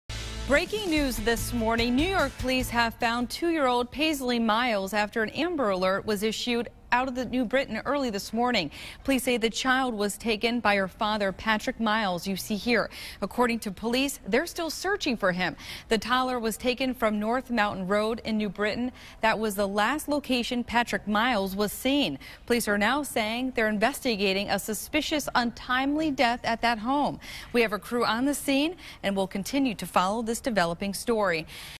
2 year-old New Britain girl found safe after Amber Alert
Police say 2-year-old Paisley Miles was found safe early this morning after an amber alert was issued for her early Saturday morning.